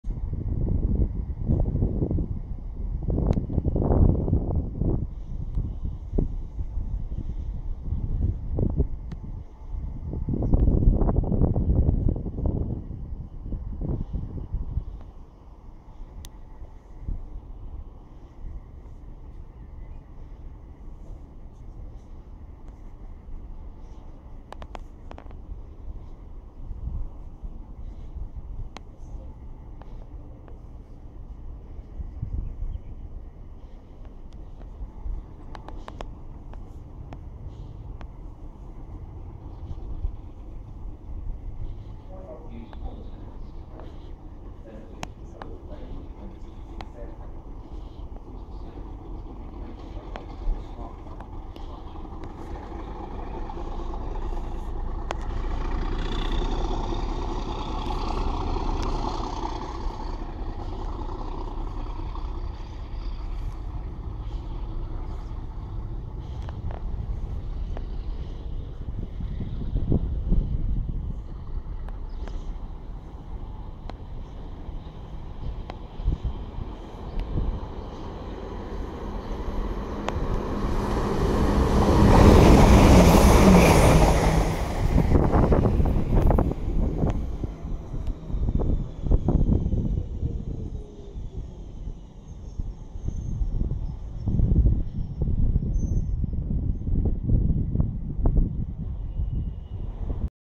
South Ruislip Train Station
Train Service From London Marylebone To High Wycombe Calling At South Ruislip